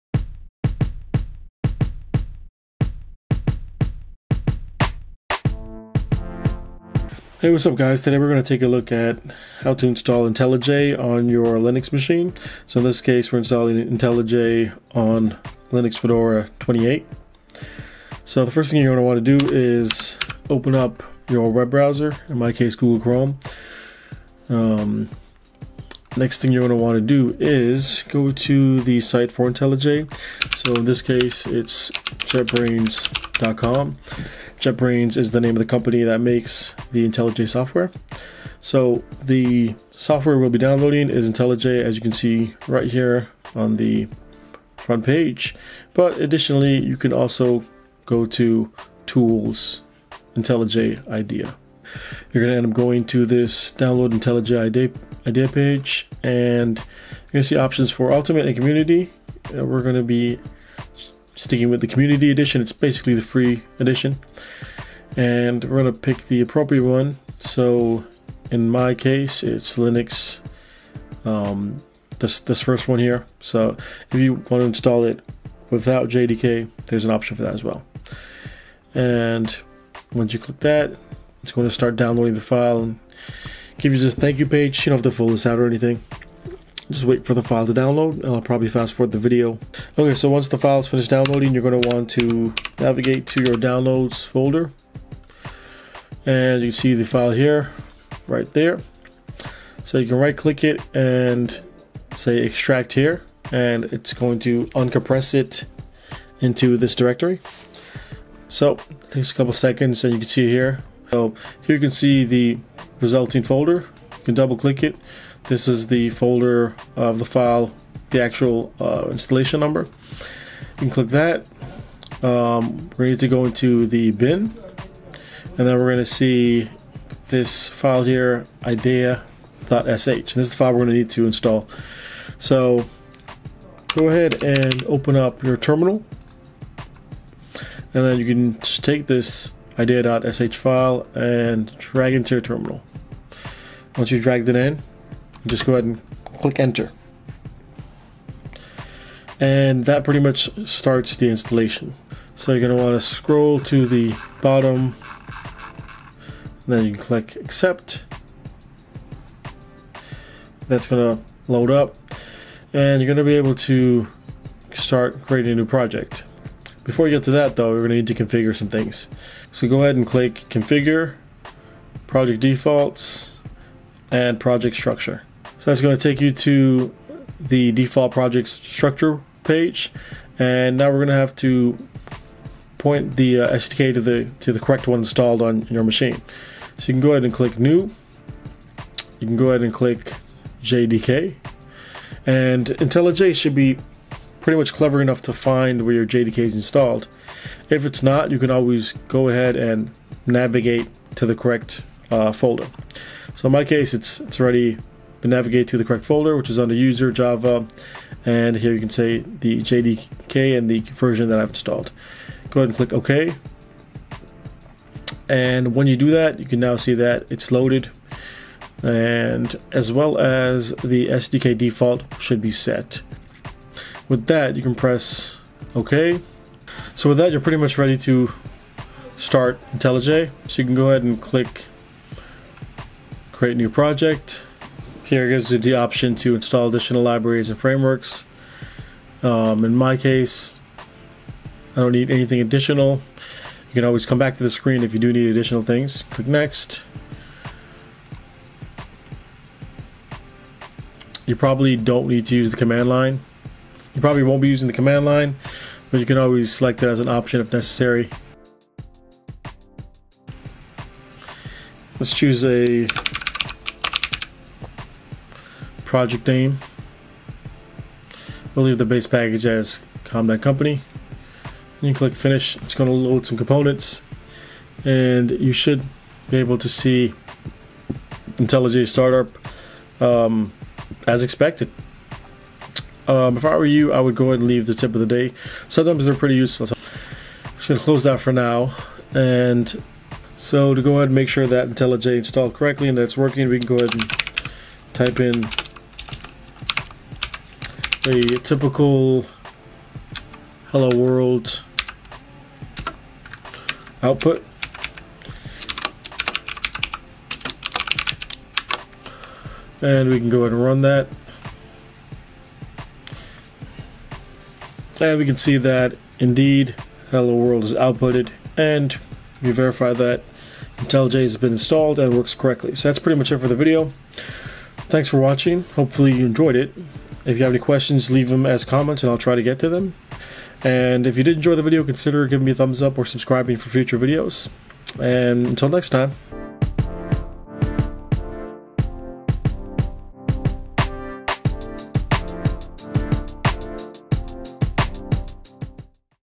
Installing IntelliJ IDEA on Linux Fedora 28
How to install IntelliJ IDEA on Linux Fedora 28.